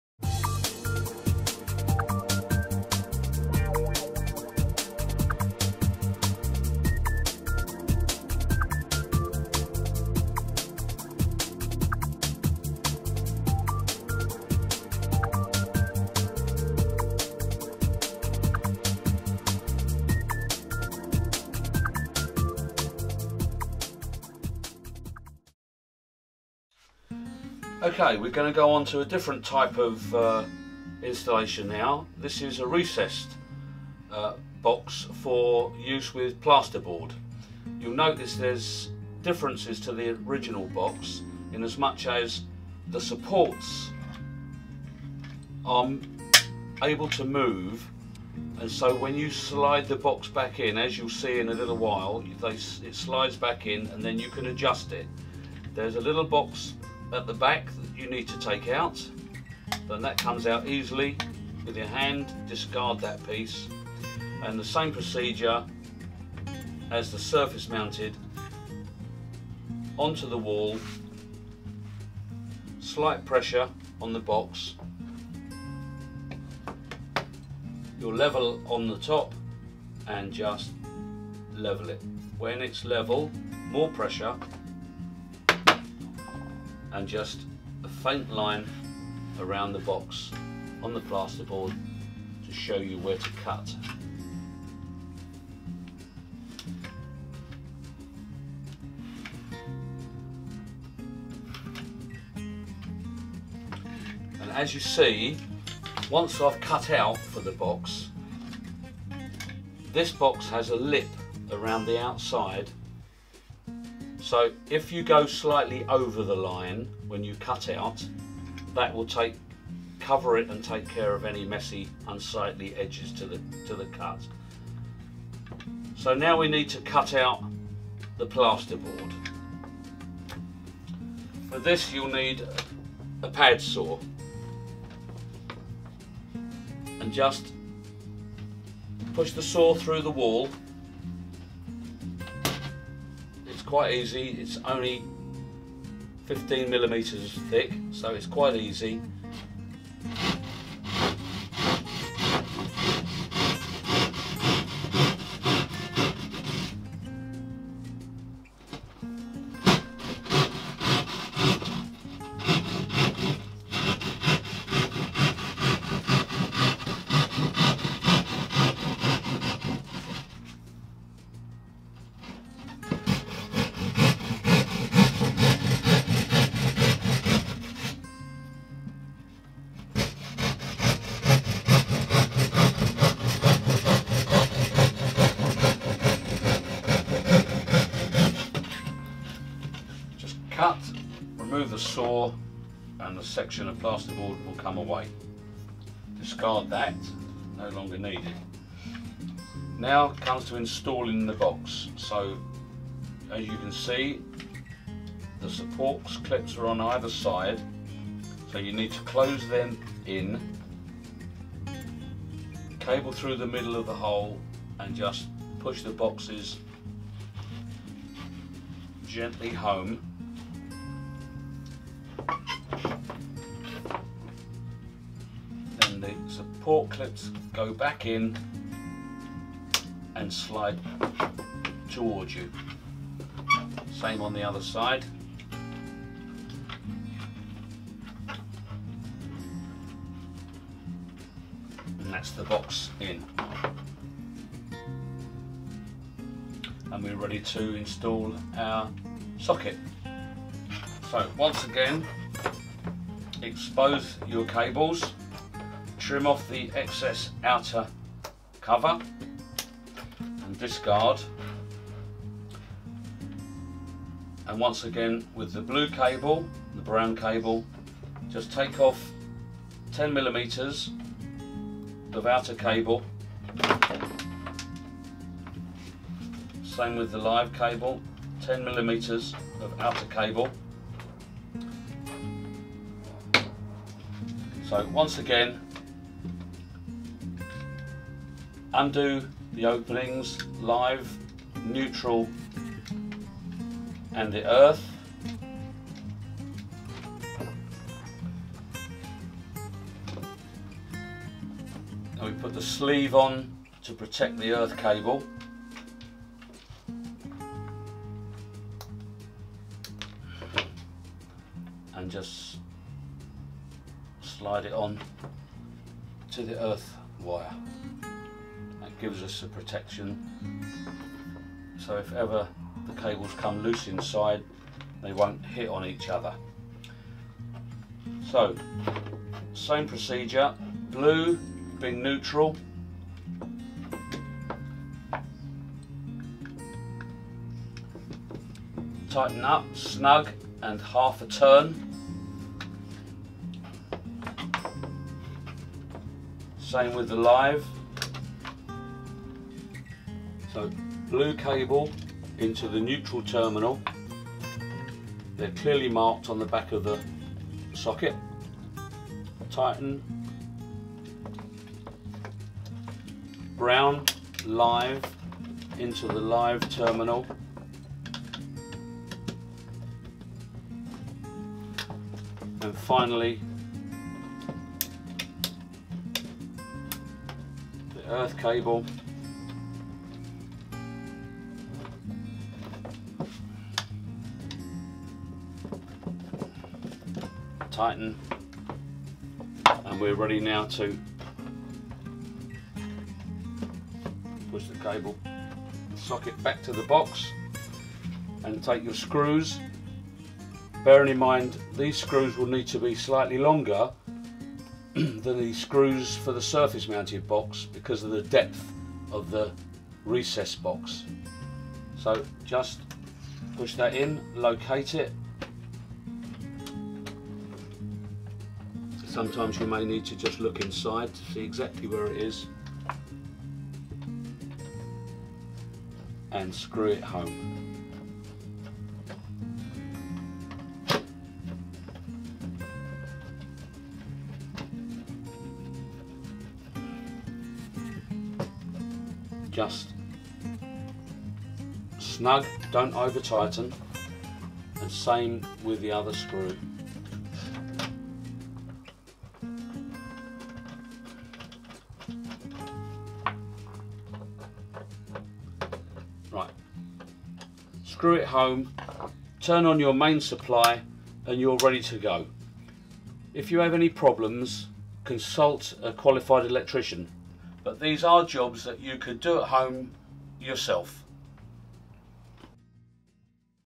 Installing a Socket (2 of 2 : Recessed-Mounted Socket)
Installing a Recessed-Mounted Socket Instructional Video

TRANSCRIPT:
OK, we're going to go onto a different type of installation now.  This is a recessed box for use with plasterboard.  You'll notice there's differences to the original box in as much as the supports are able to move so when you slide the box back in as you'll see in a little while, it slides back in and then you can adjust it.  There's a little box at the back you need to take out and that comes out easily with your hand, discard that piece.  And the same procedure as the surface mounted: onto the wall, slight pressure on the box, you level on the top and just level it.   When it's level, more pressure and just a feint line around the box on the plasterboard to show you where to cut.

As you see, once I've cut out for the box, this box has a lip around the outside so if you go slightly over the line when you cut out, that will cover it and take care of any messy unsightly edges to the cut.

So now we need to cut out the plasterboard.  For this you'll need a PADSAW.  Just push the saw through the wall, it's quite easy, it's only 15mm thick.  Just cut, remove the saw and the section of plasterboard will come away.  Discard that, no longer needed.

Now comes to installing the box.  So as you can see, the support clips are on either side.  So you need to close them in, the cable goes through the middle of the hole and just push the boxes gently home.  Then the support clips go back in and slide towards you.  Same on the other side and that's the box in. Then we're ready to install our socket.

So once again, expose your cables, trim off the excess outer cover and discard.  And once again with the blue cable, and brown cable, just take off 10mm of outer cable, same with the live cable, 10mm of outer cable.  So once again, undo the openings - live, neutral and the earth.  Put the sleeve on to protect the earth cable and just slide it on to the earth wire.  It gives us some protection so if ever the cables come loose on the side, they won't hit on each other.  So same procedure, blue being NEUTRAL - tighten up snug and half a turn.  Same with the live.  So blue cable into the NEUTRAL terminal (they're clearly marked on the back of the socket), tighten.  Brown - LIVE - into the LIVE terminal.  And finally the EARTH cable, tighten and we're ready now to push the cable and socket back to the box.

Then take your screws, bearing in mind these screws will need to be slightly longer than the screws for the surface mounted box because of the depth of the recessed box.  So just push that in, locate it.  Sometimes you may need to just look inside to see exactly where it is, then screw it home.

Just snug, don't over-tighten and the same with the other screw.  Screw it home, turn on your mains supply and you're ready to go.  If you have any problems, consult a qualified electrician but these are jobs that you could do at home yourself.